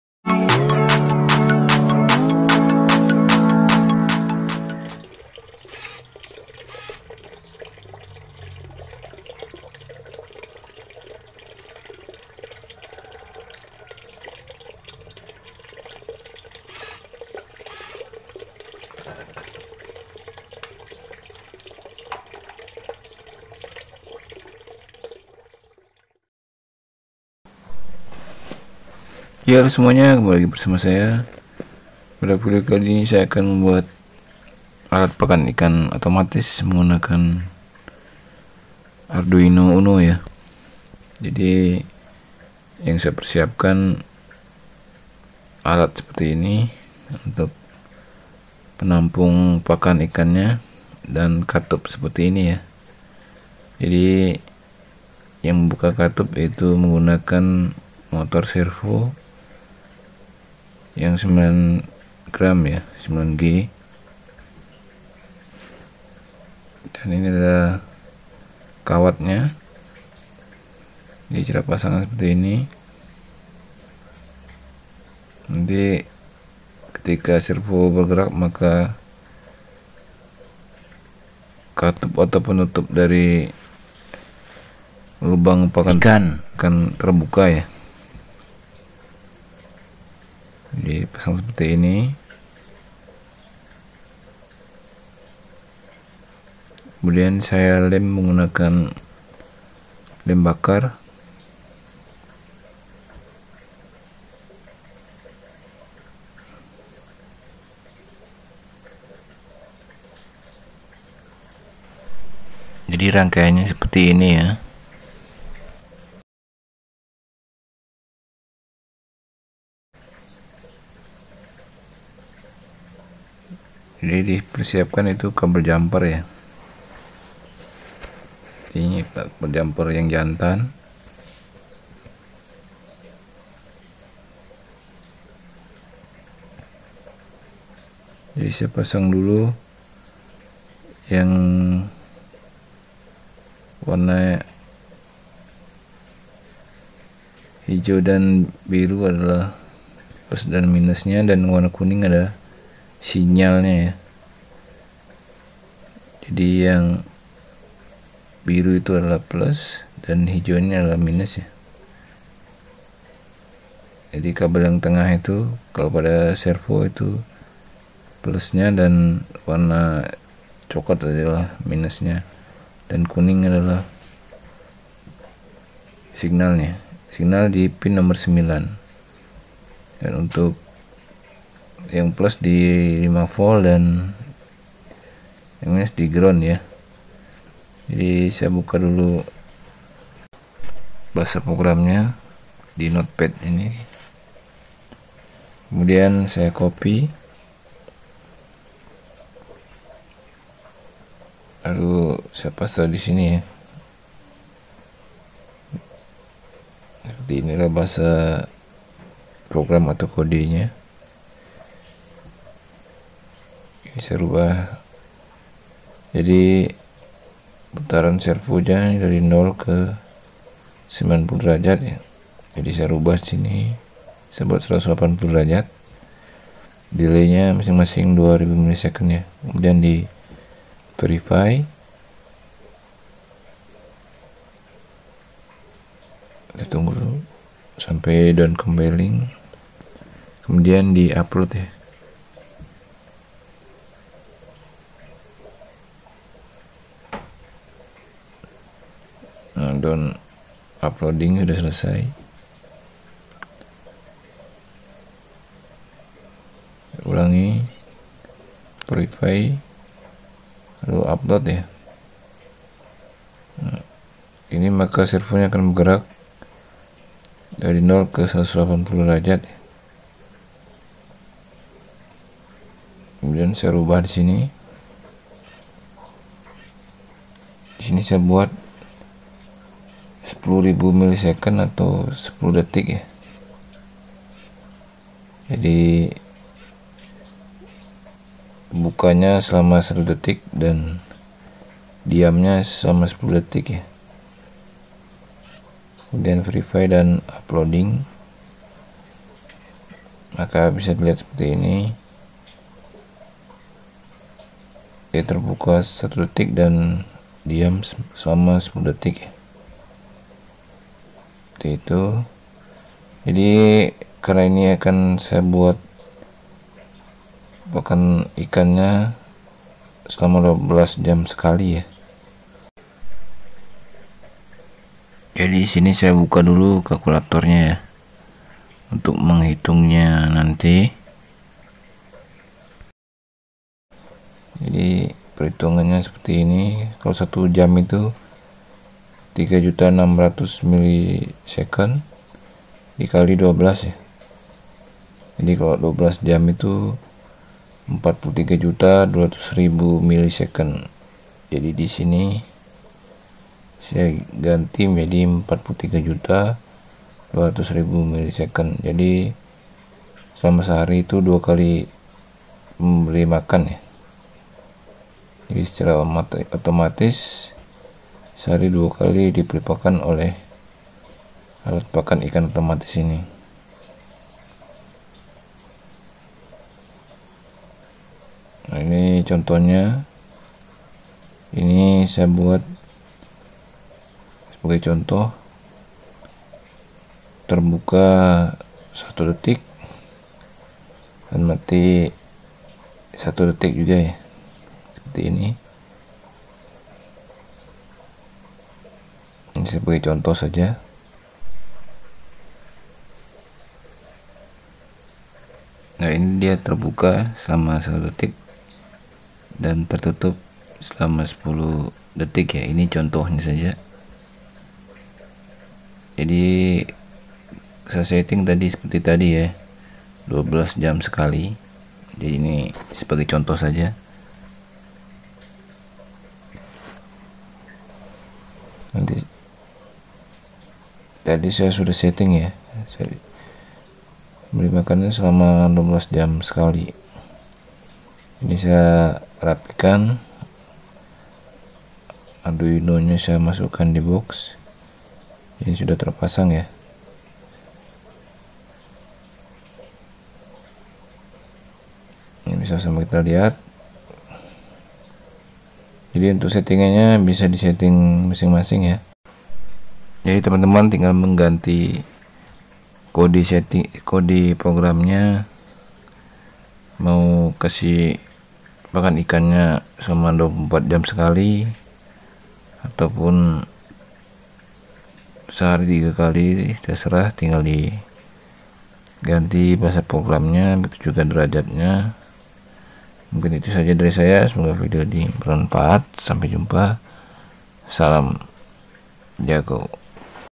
Alat Pakan Ikan Otomatis - Arduino
Alat ini sangat berguna untuk memberi pakan ikan hias kita secara otomatis baik di saat kita sibuk maupun ke luar kota dan tidak ada orang di rumah.
Bahan - bahan yang dibutuhkan adalah :
- Motor Servo
- Arduino
- Botol penampung pelet
Link untuk download kode program :